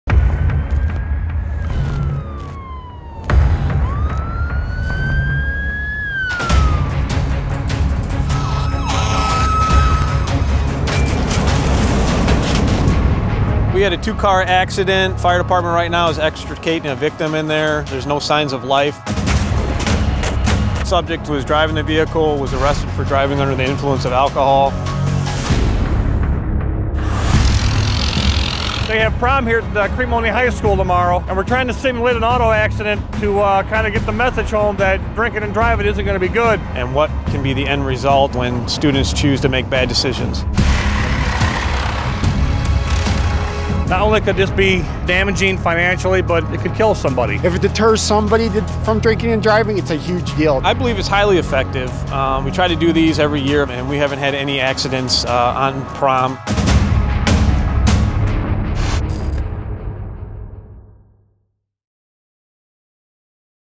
Crete-Monee High School Mock Auto Accident
Crete Fire Department and Crete Police Department conducting a mock auto accident on May 3rd at the Crete-Monee High School, to illustrate the consequences of drinking and driving, as the students prepare for prom weekend. There were no related accidents the following night.